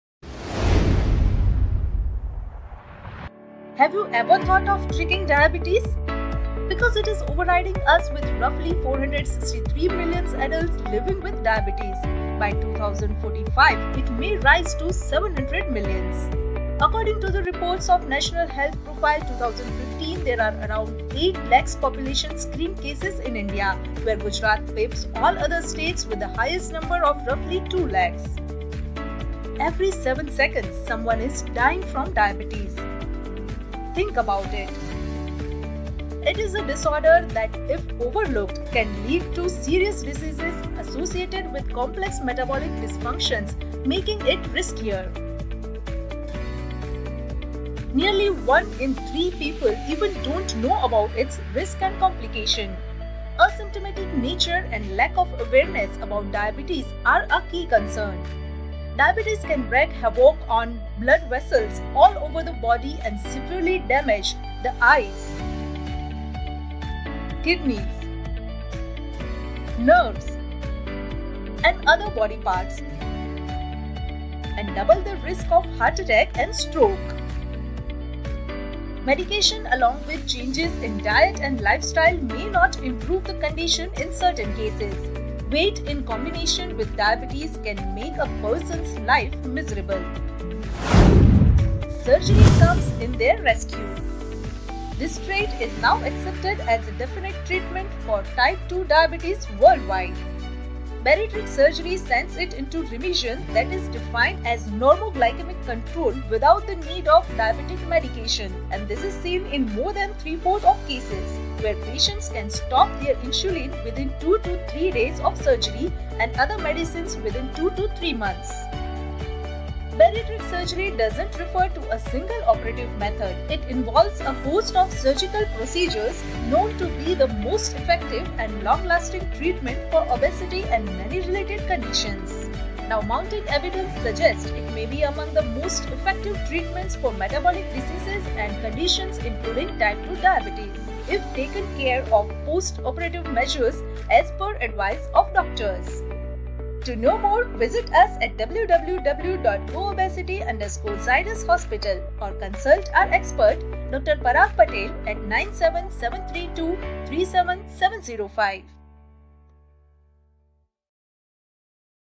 Bariatric surgery Can Cure Your Diabetes-2 | Diabetes Metabolic Surgery
Roughly 463 Million  Adults living with diabetes and by 2024 may rise to 700 million,every 7 sec someone is dying from diabetes  think about it
 it may affect your eyes, kidneys, hearts, brain and other body parts
nearly 3 in 1 people don't know risk factor of diabetes (lack of awareness is key concern). Weight in combination with diabetes can make a person's life miserable
3/4 cases patient stop their insulin within 2-3 days of surgery and other medicines in 2-3 months

 bariatric surgery is accepted as  definite treatment for  type-2 diabetes  world wide

for Details/inquiry  Regarding Bariatric/obesity surgery